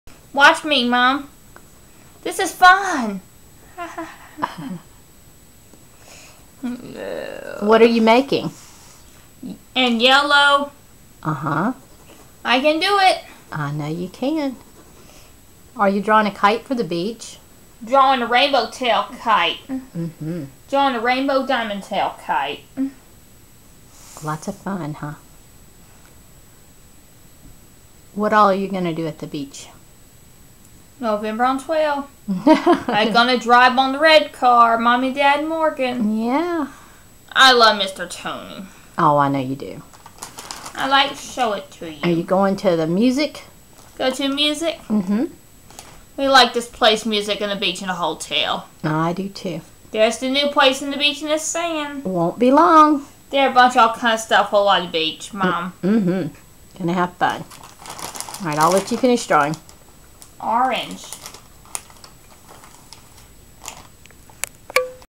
Diamond Tail Kite by Morgan Vice
Morgan is getting ready for a trip to the beach.  She's drawing the kite she plans to fly.  We will be doing a show for autism awareness on November 13th, as part of the Frank Brown International Songwriters Festival.  Drawing the kite is part of our songwriting fun.  Working with Morgan and her autism has taught me that communication is so much more than words... There's an old saying "Where words fail, music speaks."  We've learned that art is also a great communicator.